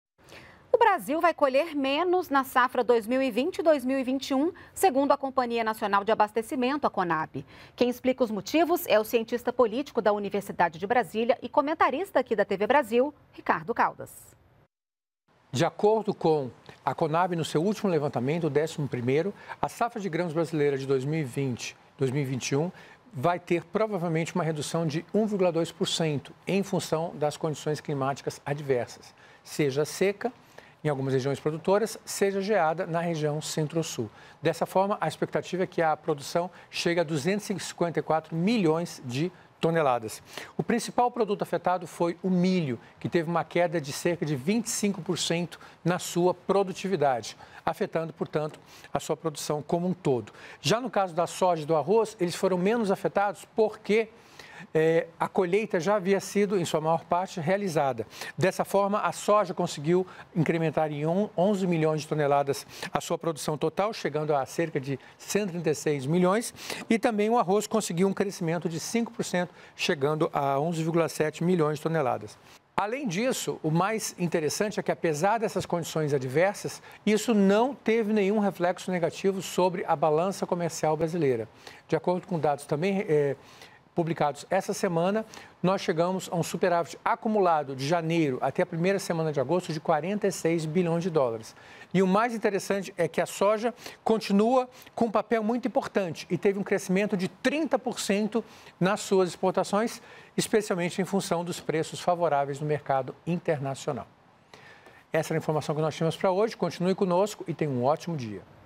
Comentário do cientista político Ricardo Caldas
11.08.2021 – O Brasil vai colher menos na safra 2020/2021, segundo a Companhia Nacional de Abastecimento, a Conab. O cientista político e comentarista da TV Brasil, Ricardo Caldas, explica os motivos.